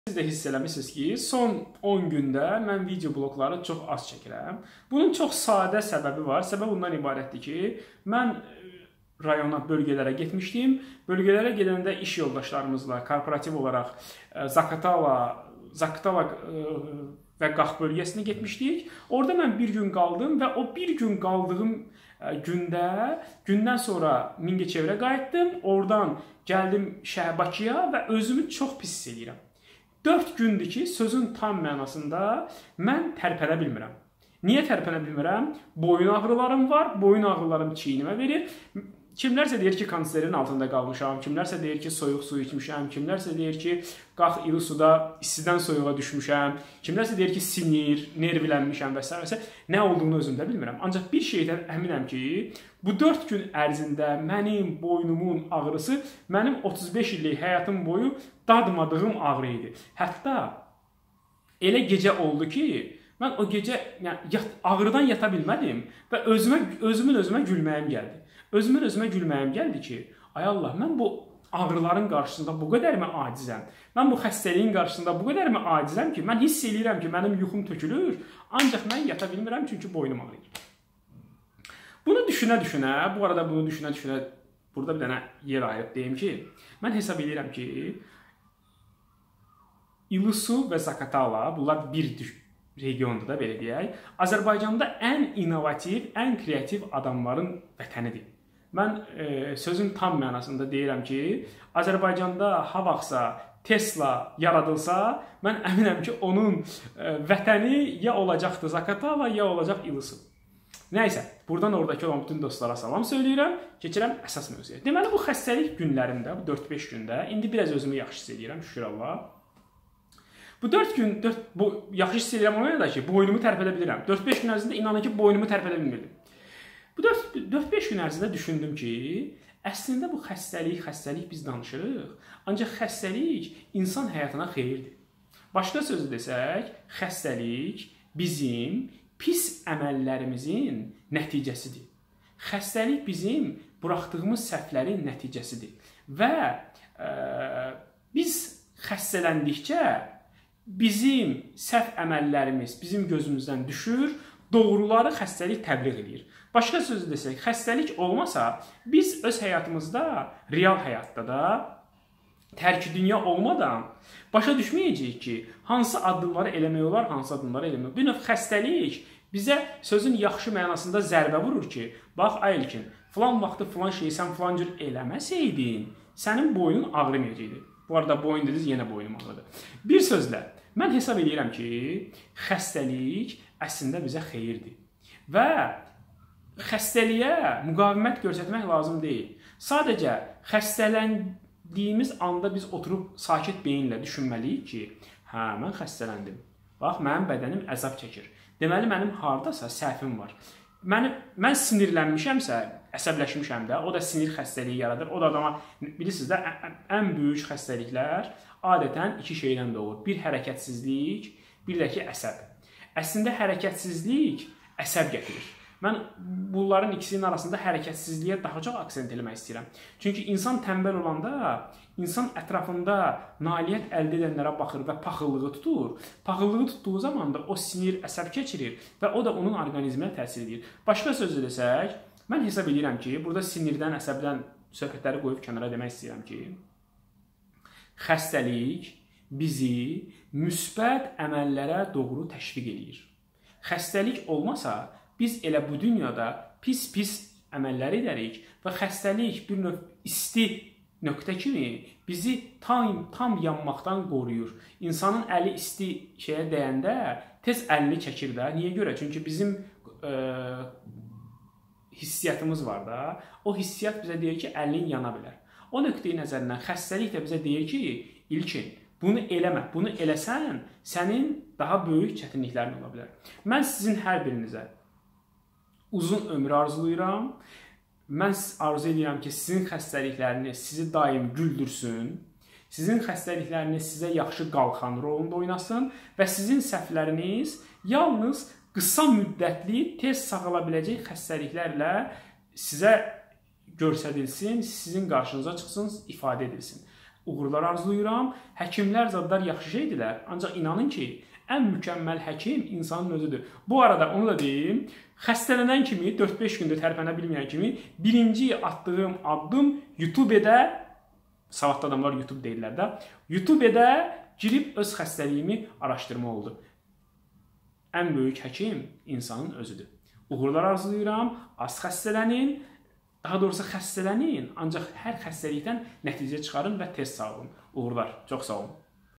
Xəstəliyin insana xeyri nədir?
Xəstəlik, ağrılar, problemlər əslində bizim üçün faydalıdır. Bilirsiz niyə?
Niyələr barədə, İlkin Manafov, video blogda misallarla izahatlar verir. Onun fikrincə xəstəlik bizim etdiyimiz pis əməllərin göstəricisidir. Xəstəlik bizi, bu dünyada səhv hərəkətlərdən qoruyur, düzgün əməllər etməyə təşviq edir.
Hər işdə olduğu kimi, bir mənfi hal varsa, deməli mütləq birdə faydalı - müsbət hal tapmaq olar.